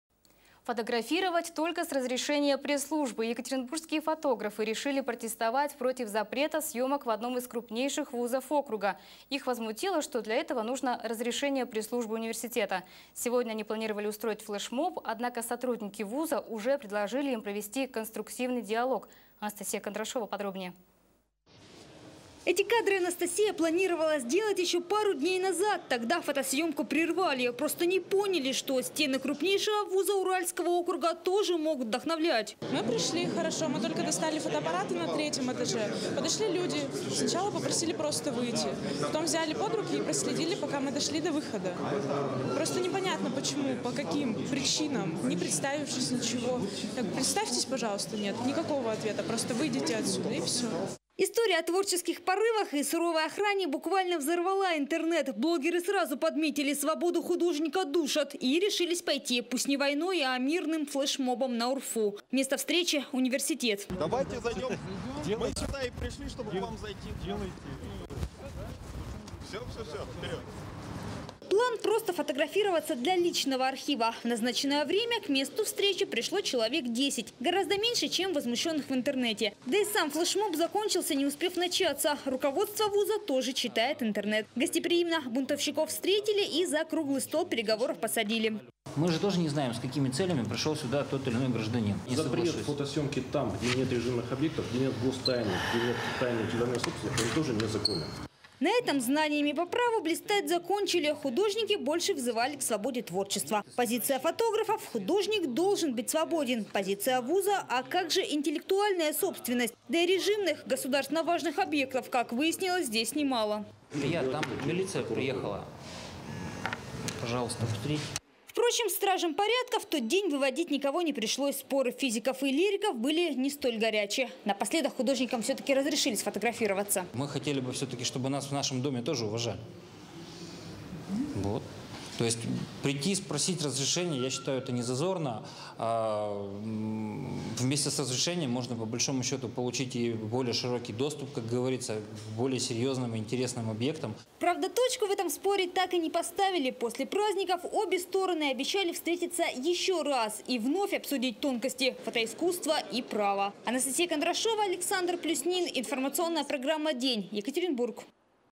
Фотографировать - только с разрешения пресс-службы
Екатеринбургские фотографы решили протестовать против запрета съёмок в УрФУ